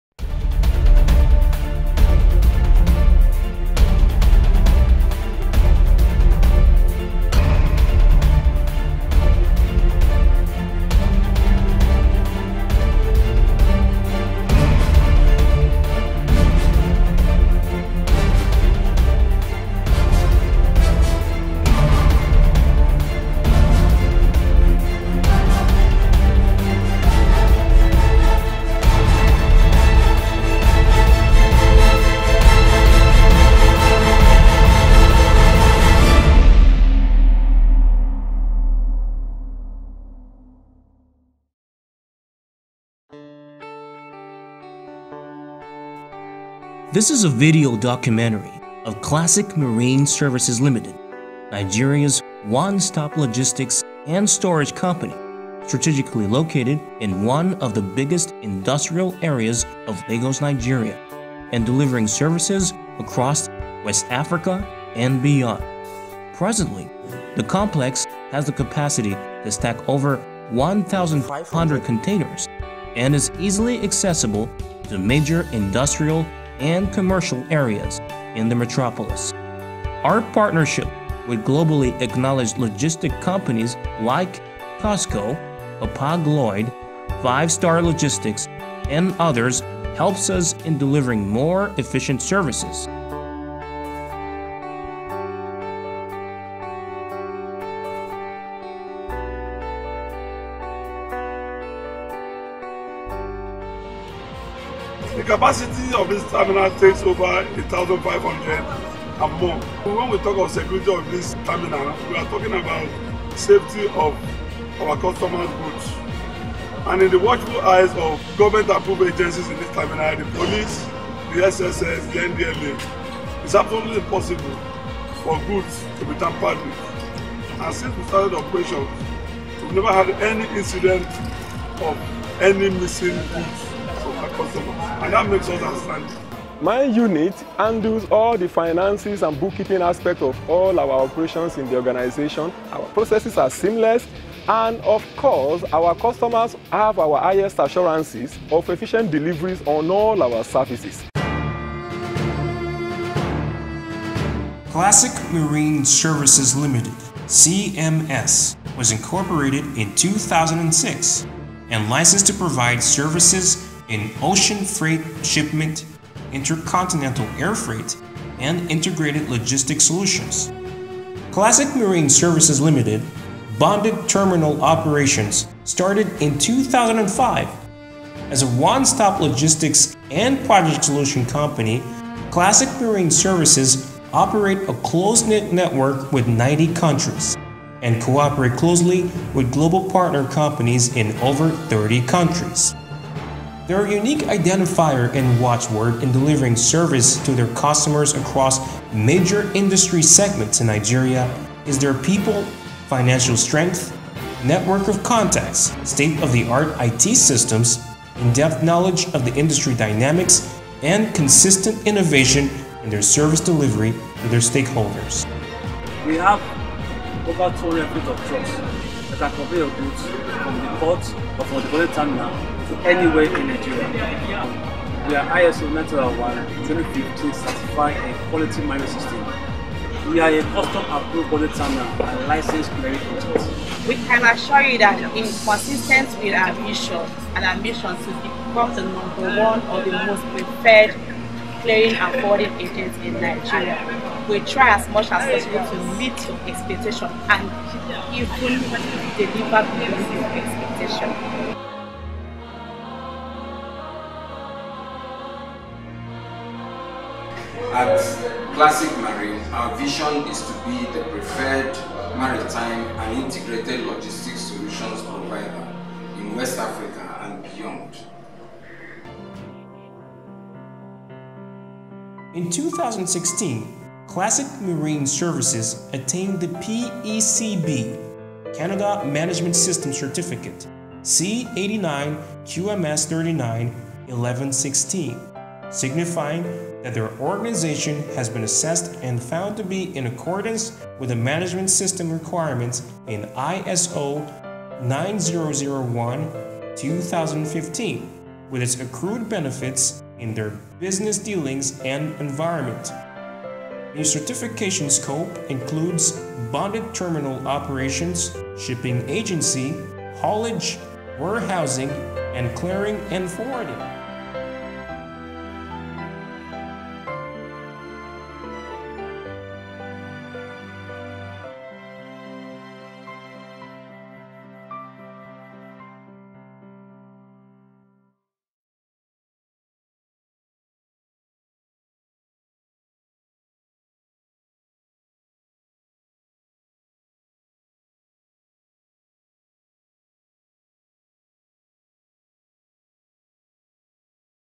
Classic Marine Services Corporate Documentary
A Corporate Documentary of Classic Marine Services Limited, a one-stop logistics and haulage company operating from Lagos Nigeria.